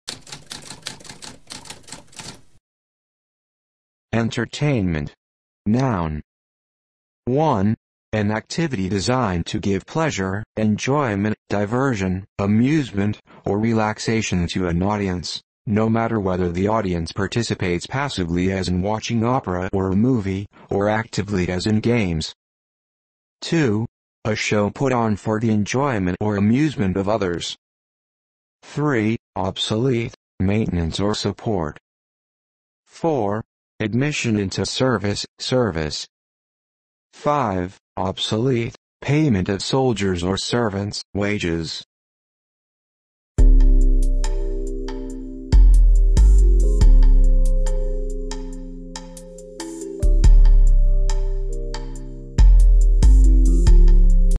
What does entertainment mean?
A spoken definition of entertainment.

Intro Sound:
Typewriter - Tamskp